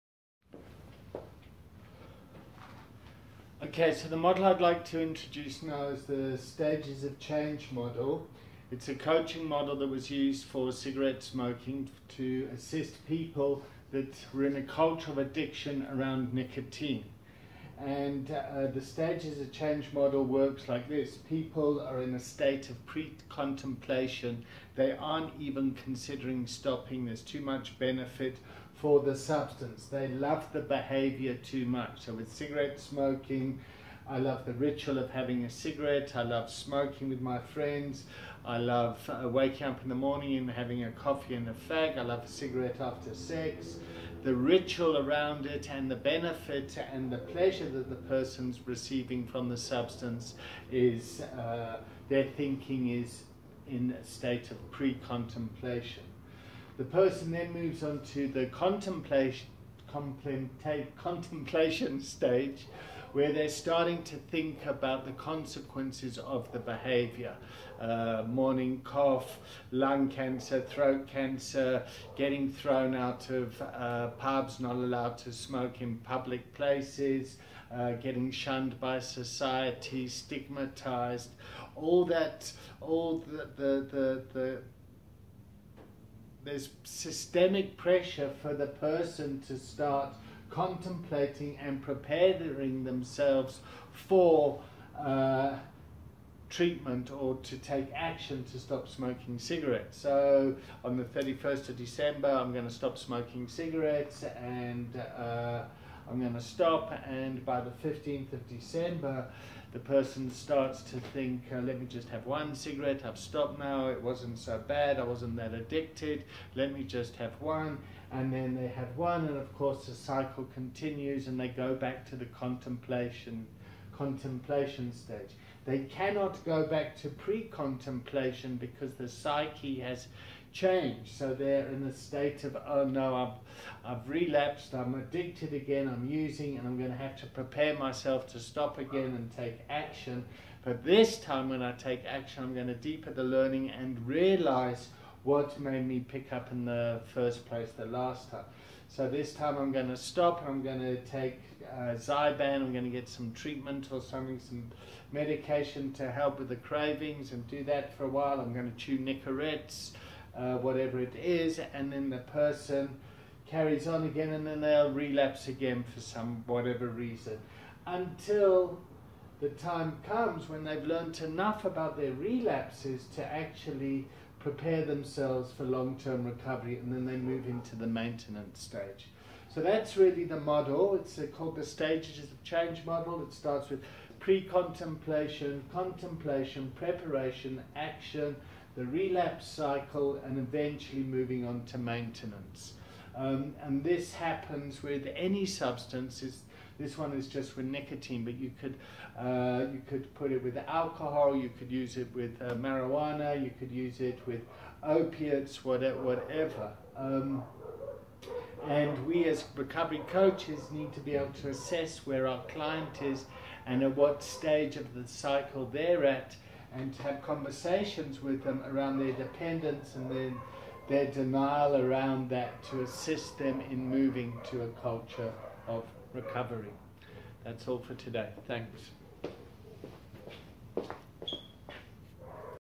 Stages of change model professional recovery coaching
Stages of change is a model that professional recovery coaches need to understand particularly useful in a culture of Addiction and harm reduction.